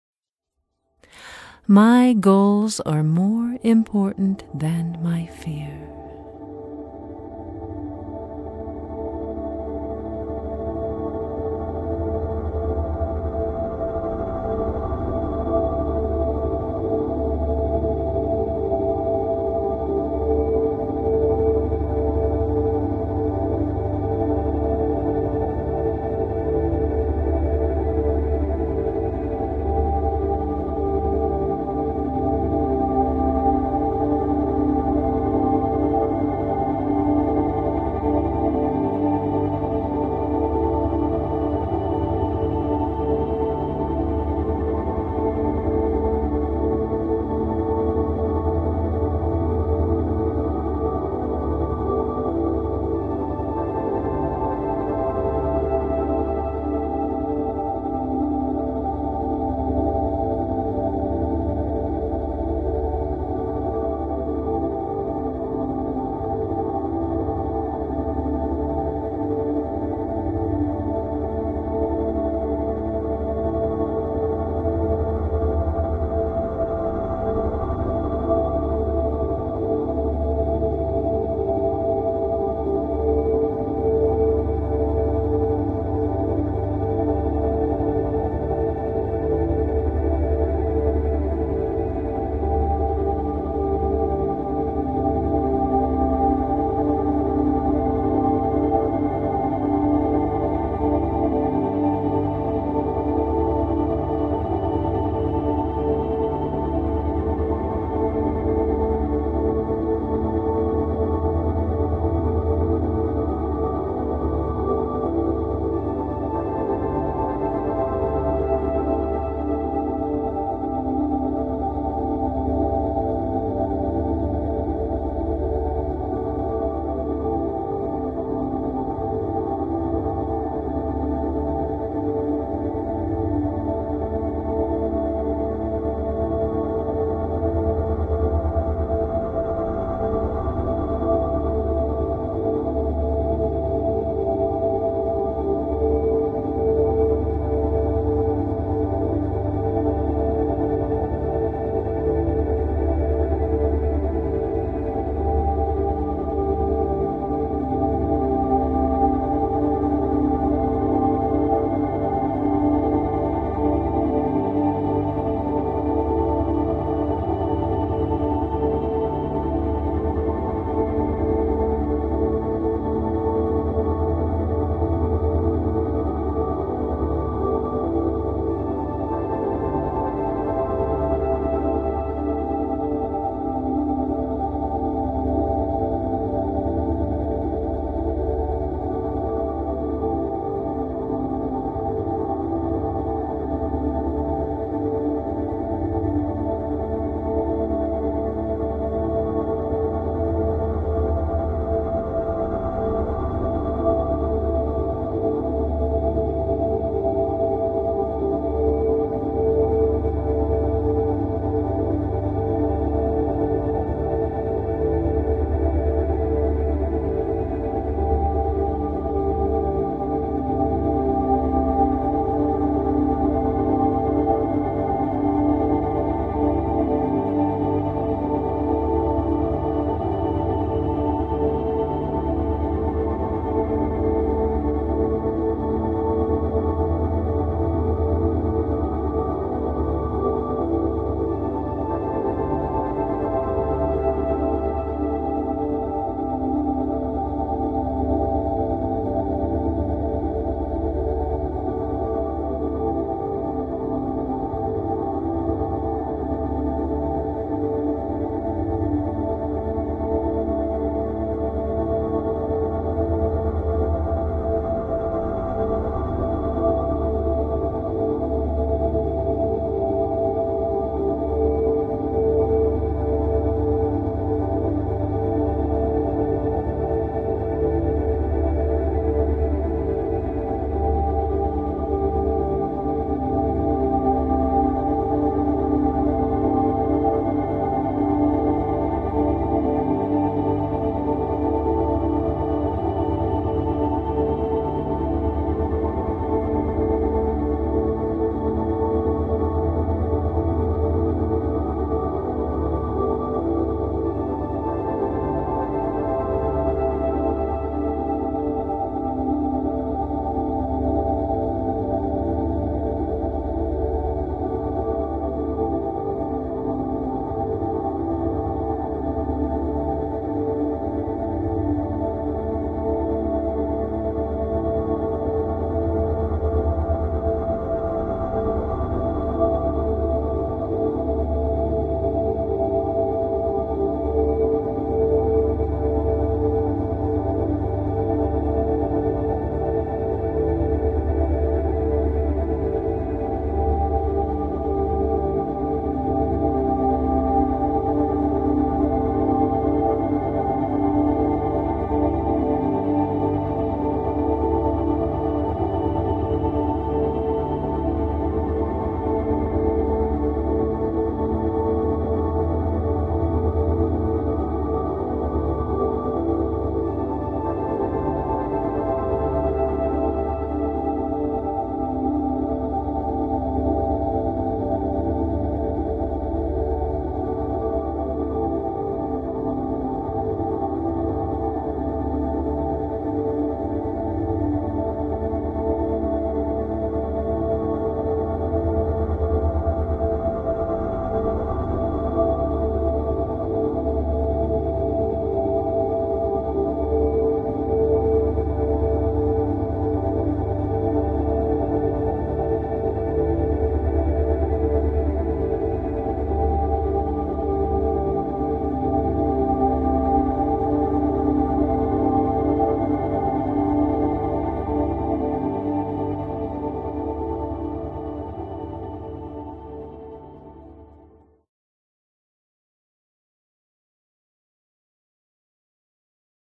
Subliminal Affirmation: Goals Outweigh Fear
Affirmation: My goals are more important than my fear.

This affirmation is read verbally once before being sped up and repeated supraliminally two hundred additional times in various formats.  For Best Results: Listen to the recording while saying the affirmations to yourself and visualizing the outcome you desire.